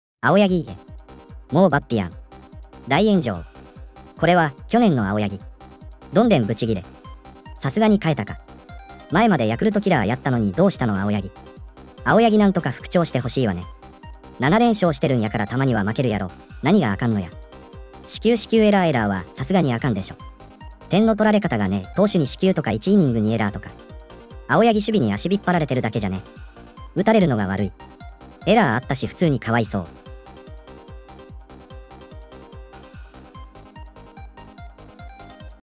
【阪神】青柳がヤクルト相手に先発した結果...【2chスレ】
-コメント-
あおやぎ、え。
もうバッピやん。
大炎上。
これは、去年の青柳。
どんでんブチギレ。
流石に変えたか。
前までヤクルトキラーやったのにどうしたの青柳。
青柳なんとか復調してほしいわね。
7連勝してるんやからたまには負けるやろ、何があかんのや。
四球四球エラーエラーは流石にあかんでしょ。
点の取られ方がね、投手に四球とか1イニング2エラーとか。
青柳守備に足引っ張られてるだけじゃね？
打たれるのが悪い。
エラーあったし普通にかわいそう。